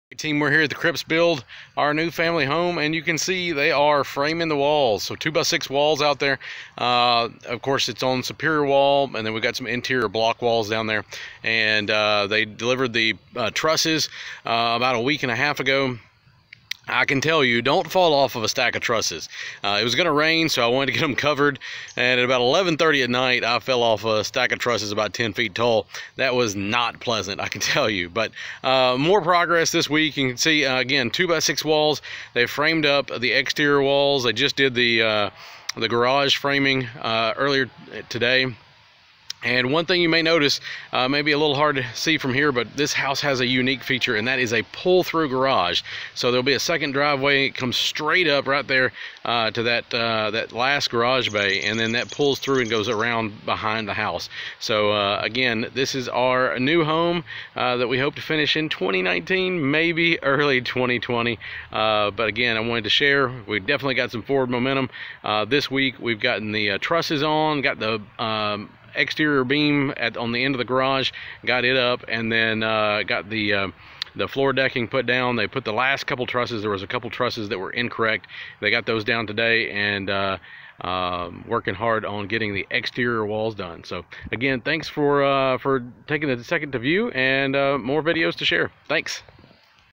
Walls going up on our New Home 2019
Framing our 2x6 walls on our custom home in Tennessee